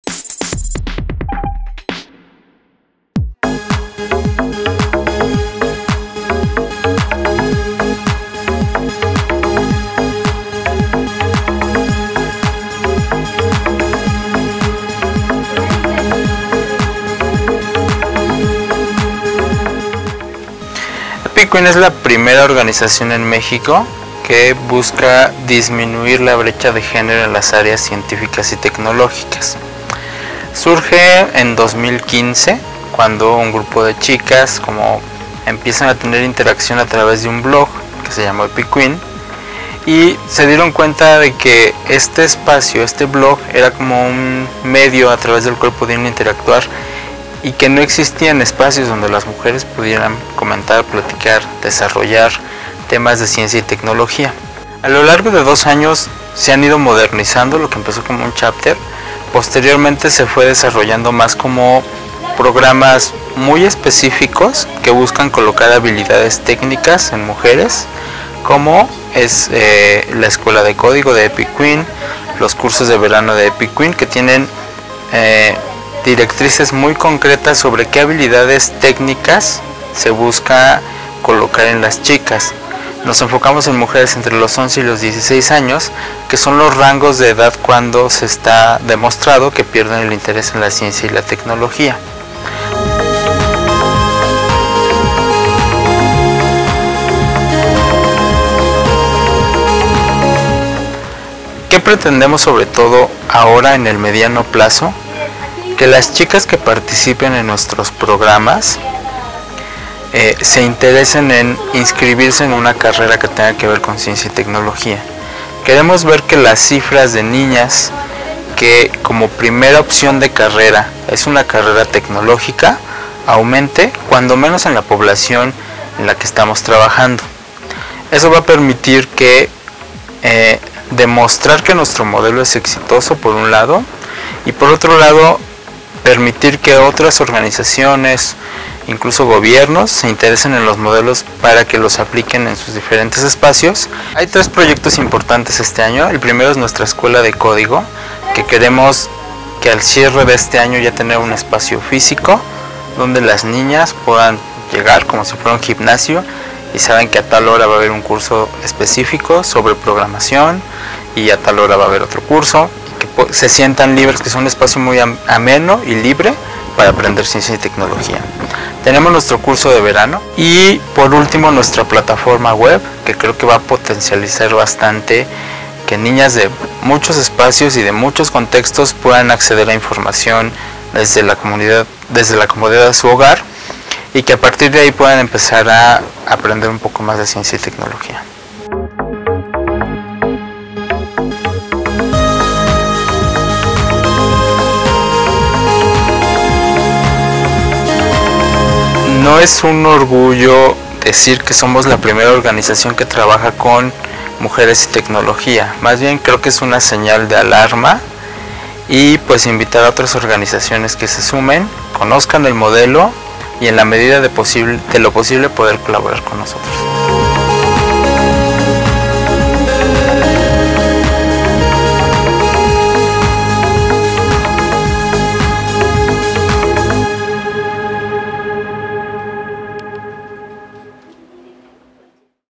Epic Queen: Mujeres y niñas en el sector científico y tecnológico
Esta organización en México busca disminuir la brecha de género a través de diferentes programas.